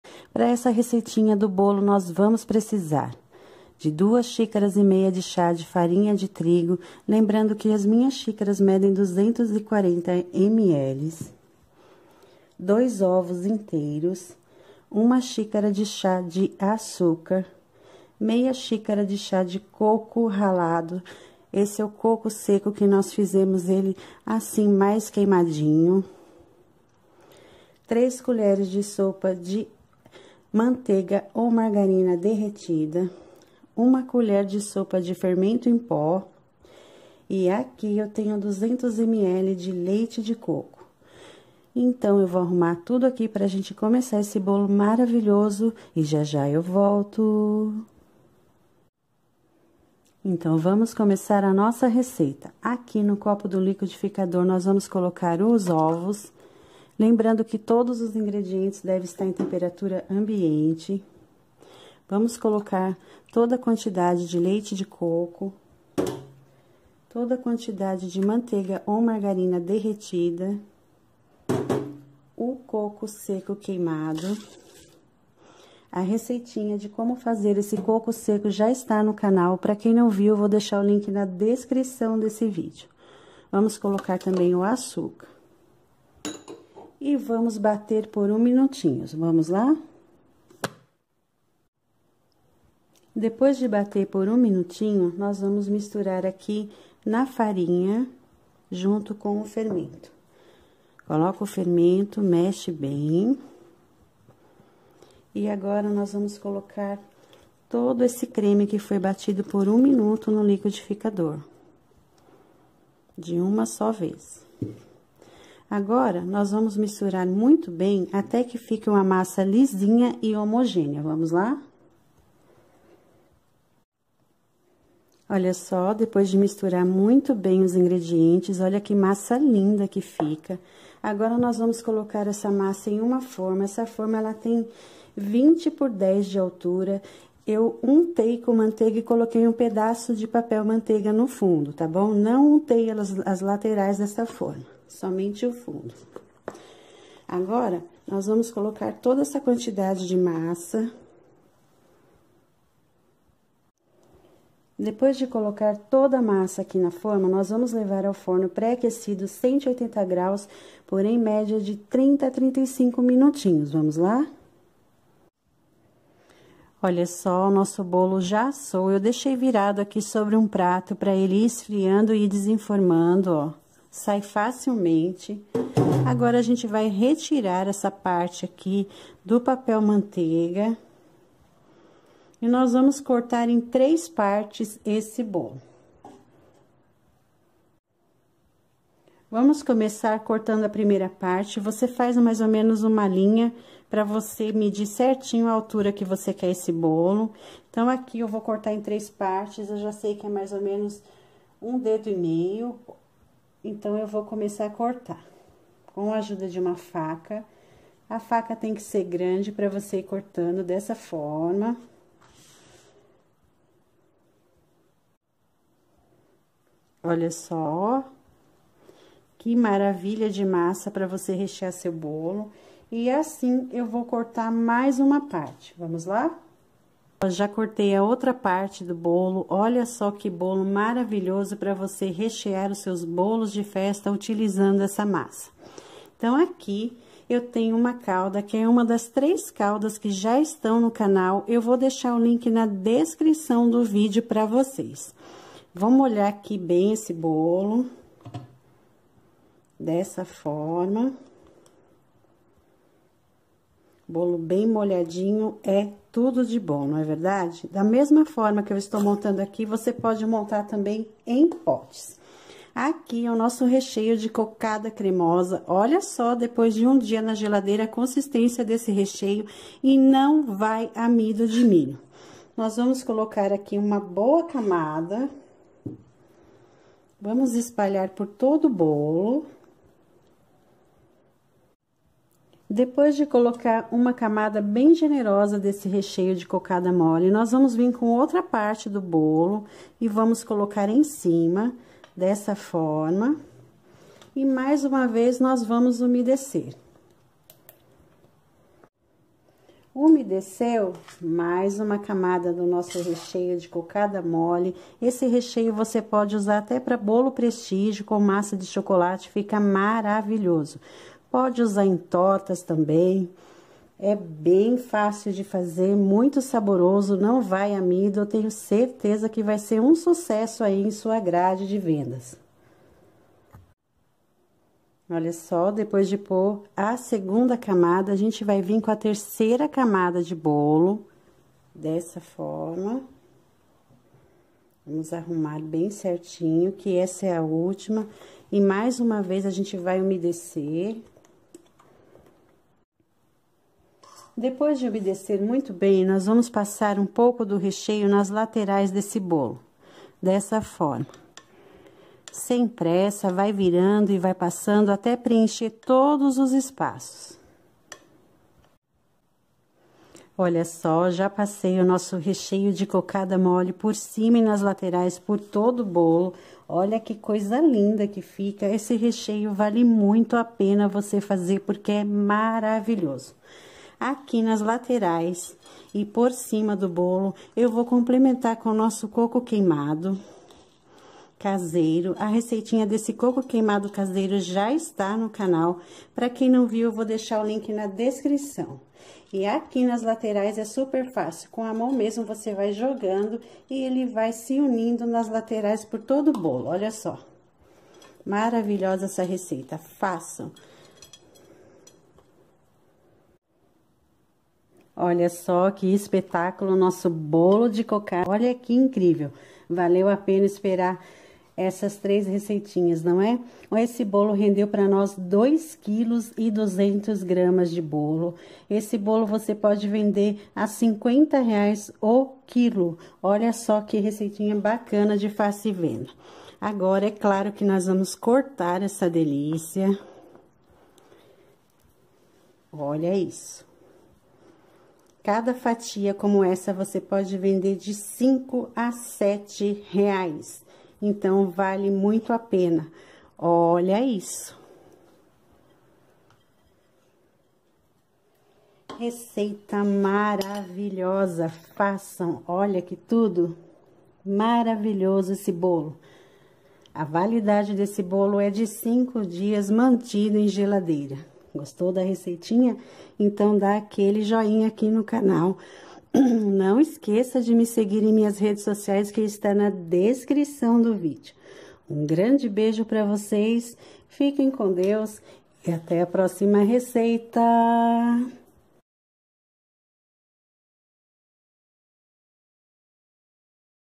BOLO DE FESTA SEM PRENSAR/MASSA E RECHEIO FÁCIL DE FAZER VEJAM 😱 BOLO de coco QUEIMADO.
Obrigada a todos pelo carinho bjs 😘

🔹Fan Page ➡️ Nicia Trufas & Balas Geladas

Ingredientes ⬇️
Uma receita inteira do recheio de Cocada Cremosa link acima 👆✔️
Uma receita inteira do coco queimado link do vídeo acima 👆✔️
Duas xícaras e meia chá de farinha de trigo ✔️
Dois ovos inteiros ✔️
Uma xícara de chá de açúcar ✔️
Uma colher de sopa de fermento em pó ✔️
Três colheres de sopa de manteiga ou margarina derretida ✔️
Meia xícara de chá de coco seco queimado ✔️
200ml de leite de coco ✔️
Modo de fazer ✔️ Vamos ver o vídeo 😘